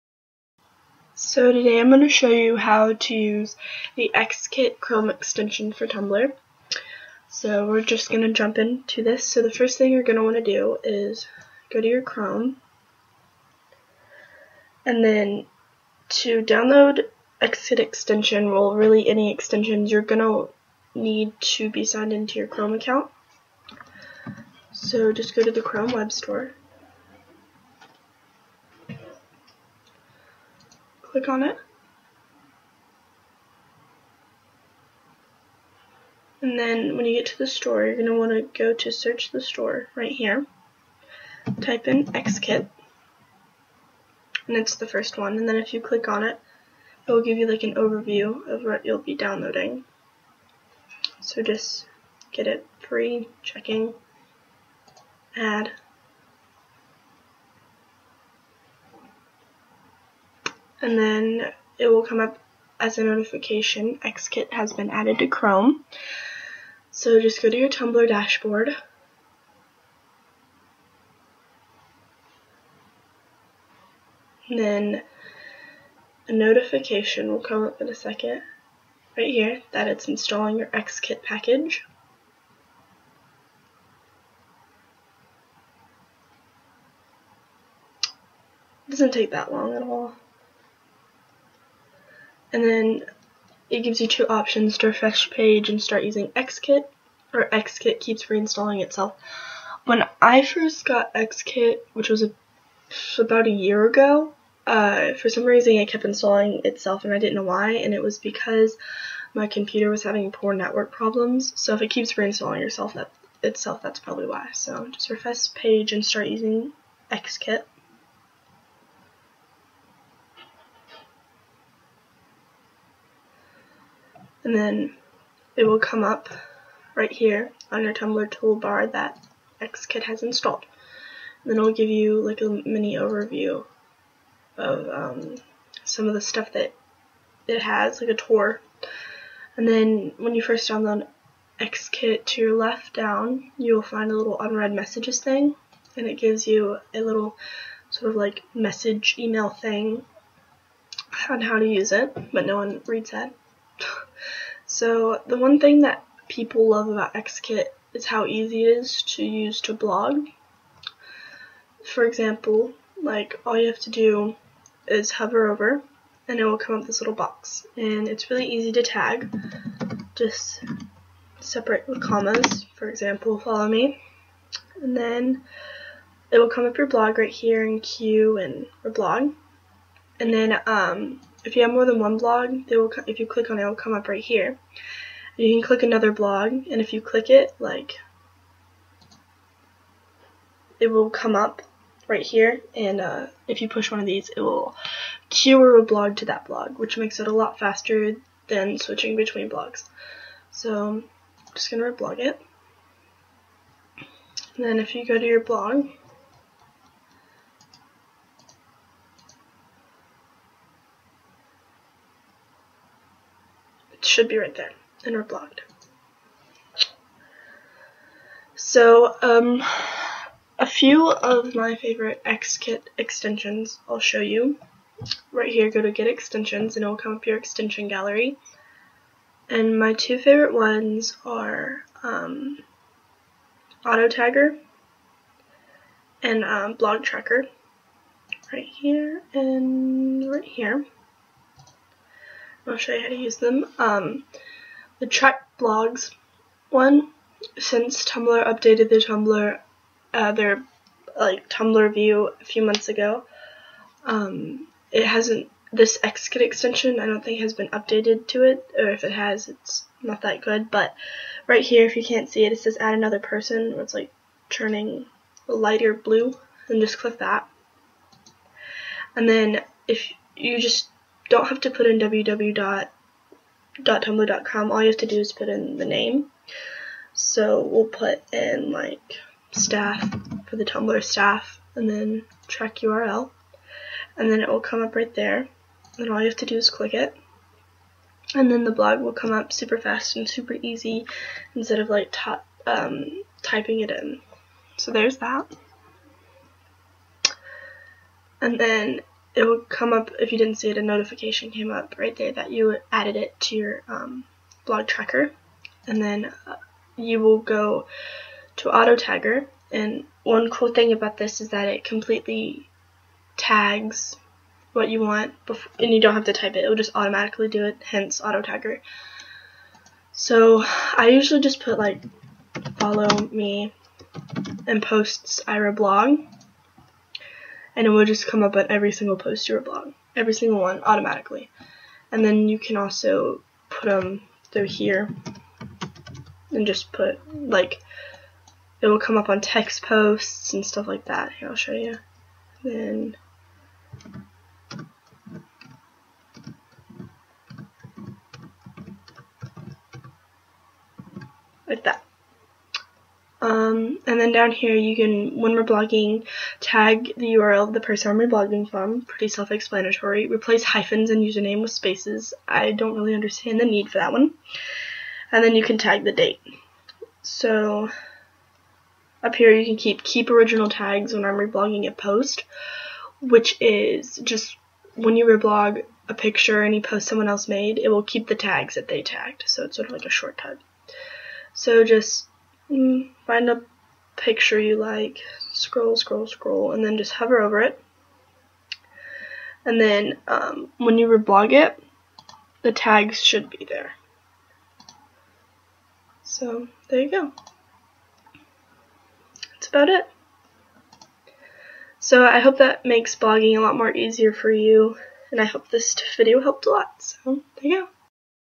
How to download and use XKIT chrome extension!
This is a simple video on how to download and use XKIT! I showed how too use two extensions, AutoTagger, and BlogTracker. It's really simple and I hope this video helps!